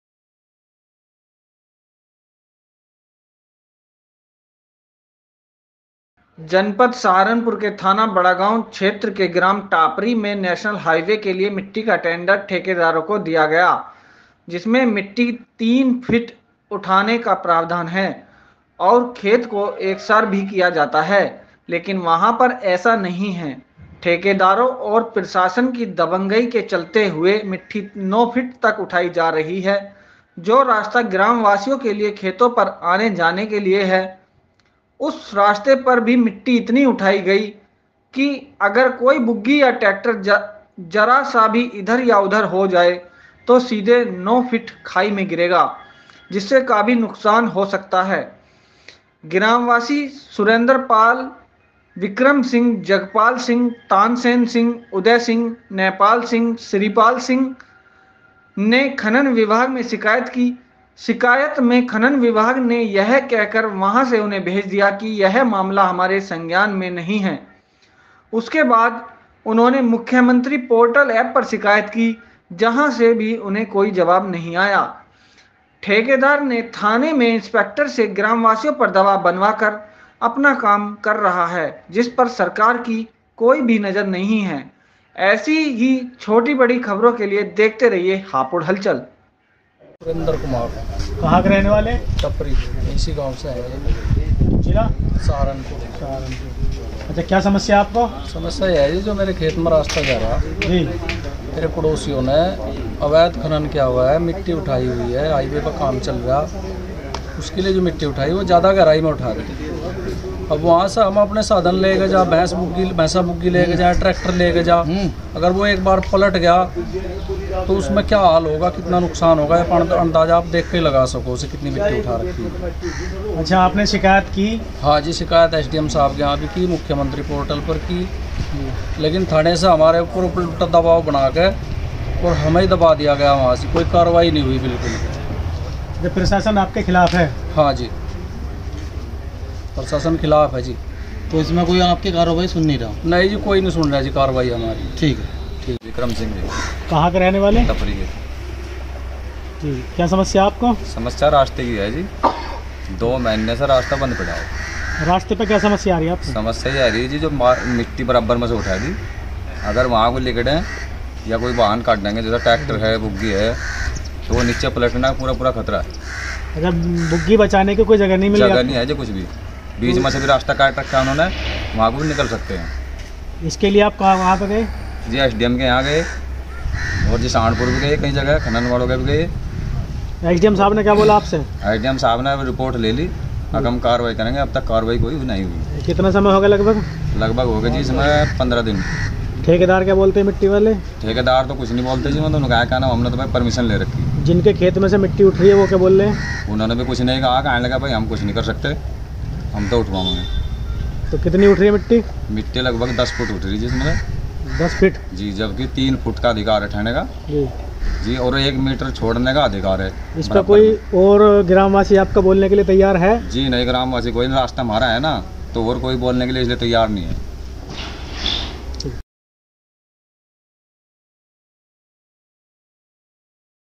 मिट्टी उठाई जाने से ग्राम वासियों को रास्ते में आने जाने हो रही हैं परेशानी | Hapur Hulchul
मिट्टी उठाई जाने से ग्राम वासियों को रास्ते में आने जाने हो रही हैं परेशानी

जनपद सहारनपुर के थाना बड़ागांव क्षेत्र के ग्राम टापरी में नेशनल हाईवे के लिए मिट्टी का टेंडर ठेकेदारों को दिया गया है जिसमें मिट्टी तीन फीट उठाई जाती है और खेत को एक साथ किया जाता है लेकिन वहां पर ऐसा नहीं है ठेकेदारों और प्रशासन की दबंगई के चलते हुए मिट्टी 9 फीट तक उठाई जा रही है जो रास्ता ग्राम वासियों के लिए खेतों पर आने जाने के लिए है उसे रास्ते पर भी मिट्टी इतनी उठाई गई है की अगर कोई  बुग्गी या ट्रैक्टर अगर जरा सी भी इधर-उधर हो जाए तो सीधे   9 फिट खाई में गिरेगा जिससे काफी नुकसान होगा ग्राम मासी सुरेंद्र पाल विक्रम सिंह जगपाल सिंह तानसेन सिंह उदय सिंह नेपाल सिंह श्रीपाल सिंह में खनन विभाग में शिकायत की शिकायत में खनन विभाग में यह कहकर वहां से उन्हें भेज दिया कि यह मामला हमारे संज्ञान में नहीं है उसके बाद वह मुख्यमंत्री पोर्टल ऐप पर शिकायत करते हैं जहां से कोई भी जवाब नहीं आया ठेकेदार ने थाने में इंस्पेक्टर से ग्राम वासियों पर दबाव बनाकर अपना काम कर रहा है जिस पर सरकार की कोई भी नजर नहीं हैVideo Maked by :-
Team :- HHN
Edit By -Zubaidali